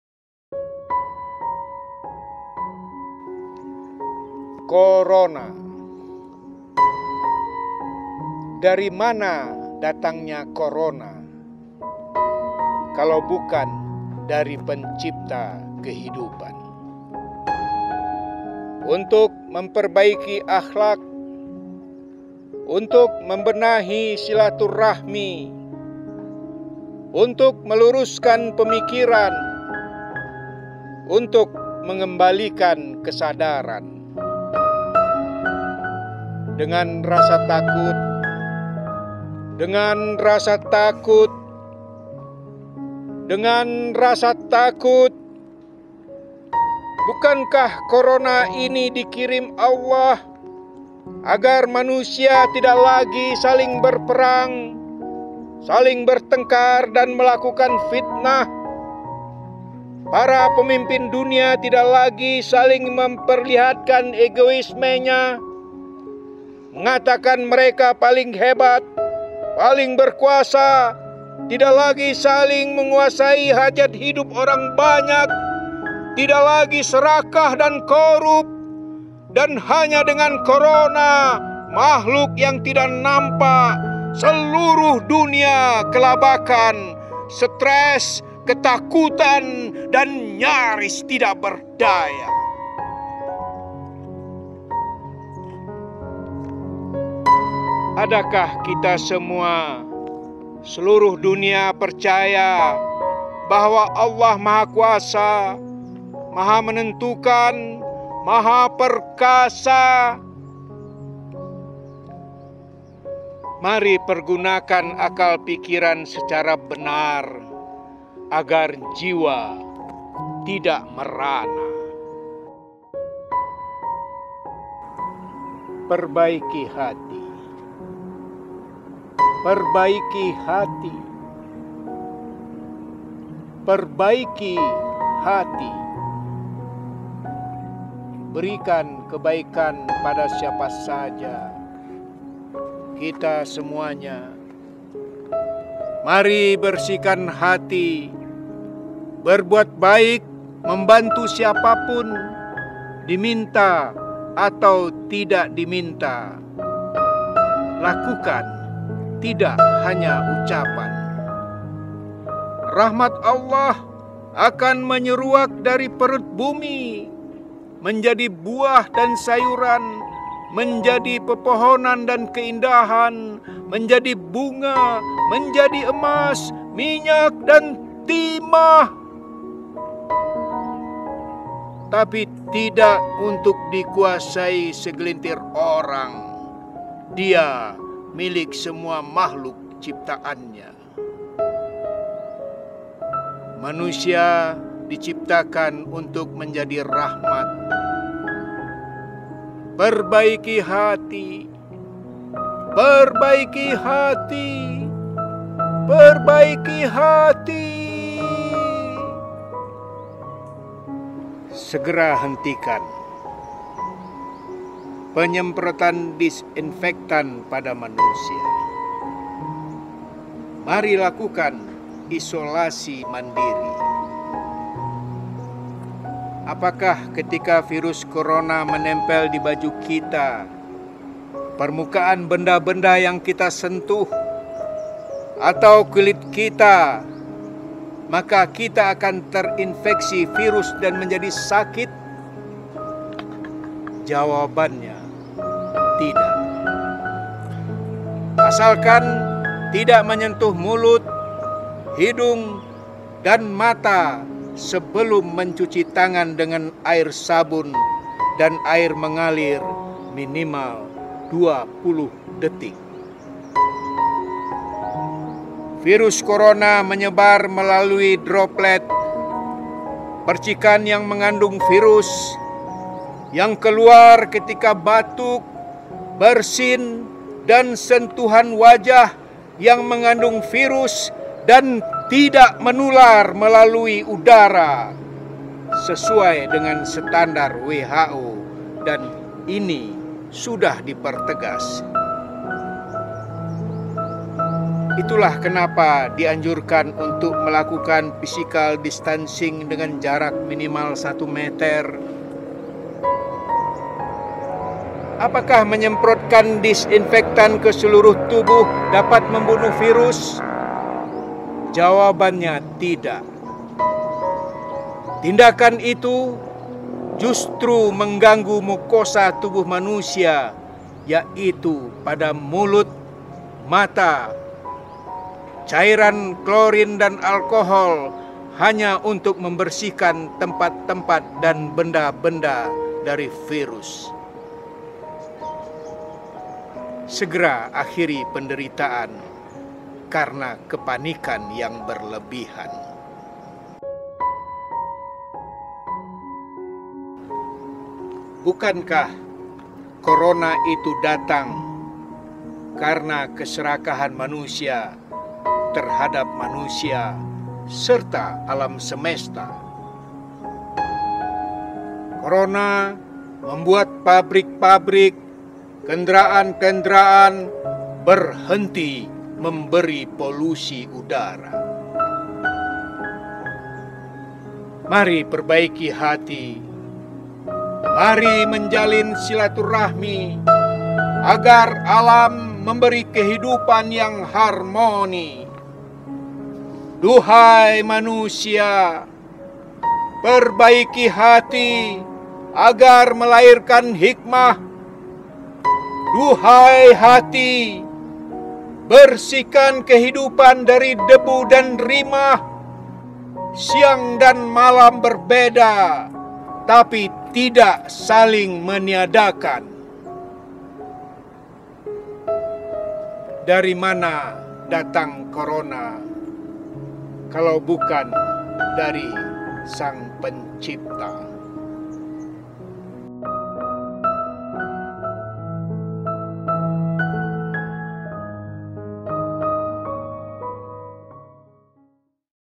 Puisi Kolaborasi BPNB Aceh dan Penyair Din Saja
Halo sobatbudaya mari kita bersama menikmati lantunan Puisi dari penyair Din Saja yang bertemakan Corona, semoga sobatbudaya yang dirumahaja sehat selalu yaa